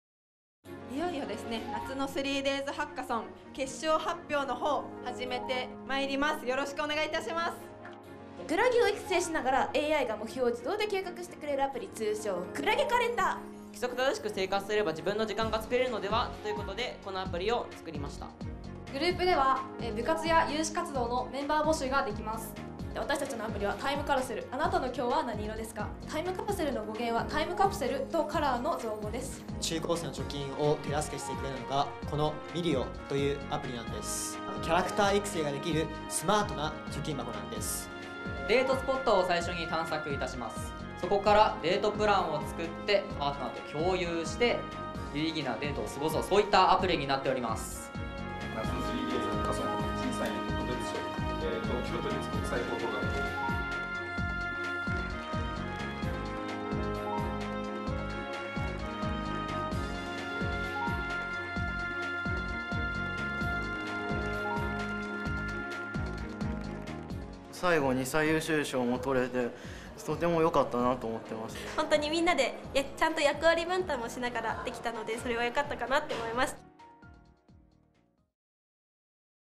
【手話版】夏の3Daysハッカソン 青春の問題をITで解決しよう！(令和5年8月31日 東京デイリーニュース No.442）
都政の情報を動画で発信しています。

NO.442は、「夏の3Daysハッカソン　青春の問題をITで解決しよう！」についてお伝えします。
都立学校の中学生・高校生などが、青春の問題を解決するアプリの企画・制作に挑戦しました。
イベントには、16グループ、約60名が参加。
IT専門家による審査の結果、３チームが表彰されました。